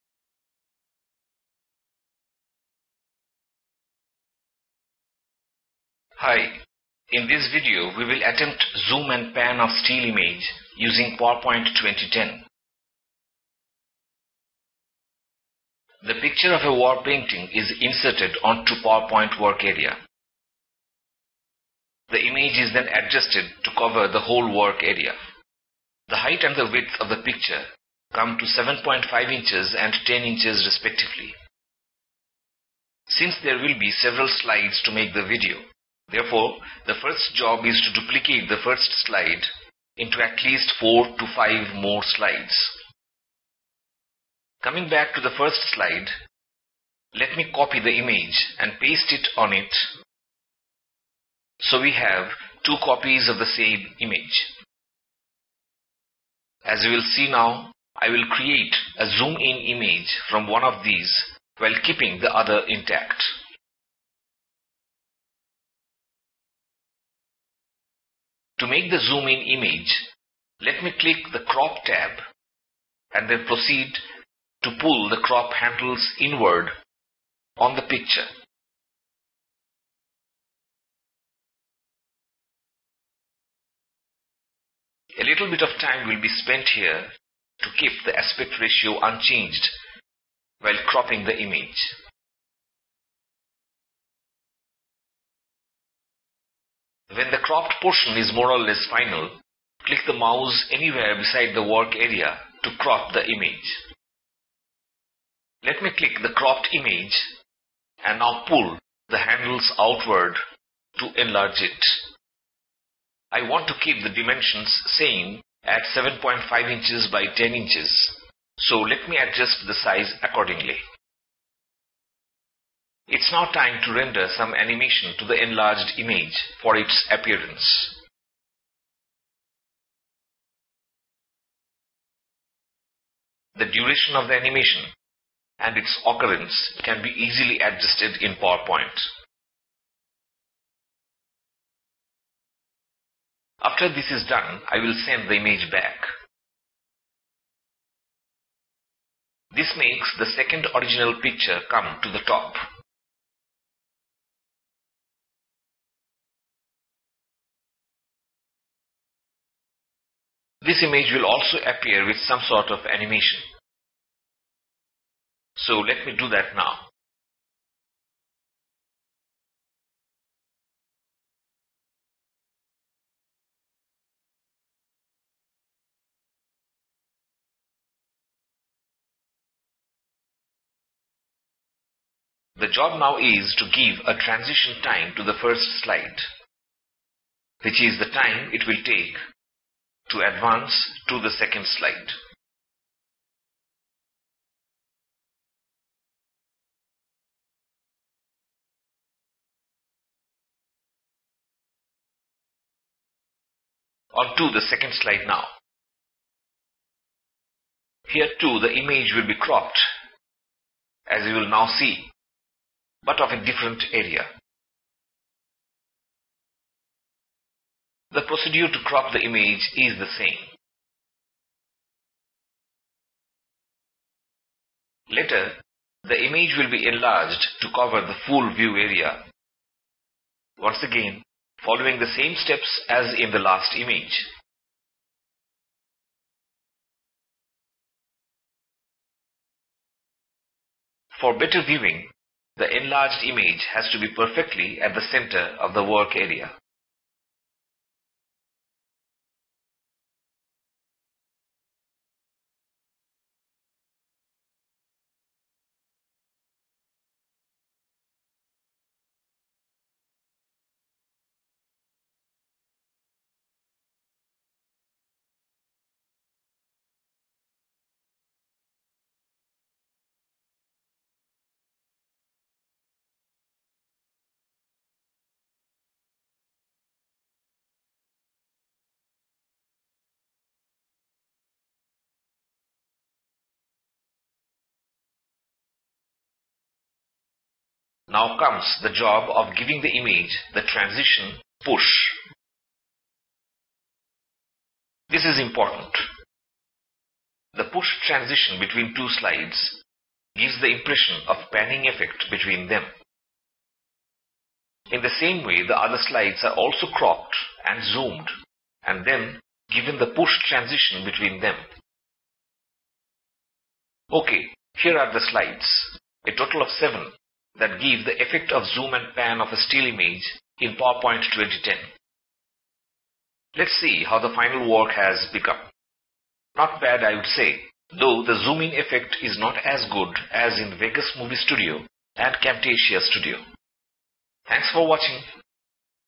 PowerPoint 2010 For Zoom & Pan Ken Burns Video Effect
This video takes a look at how to do zooming and panning to still image in PowerPoint 2010. This is the third video tutorial for Ken Burns Effect in my blog, the earlier ones being with Vegas Movie Studio and Camtasia Studio.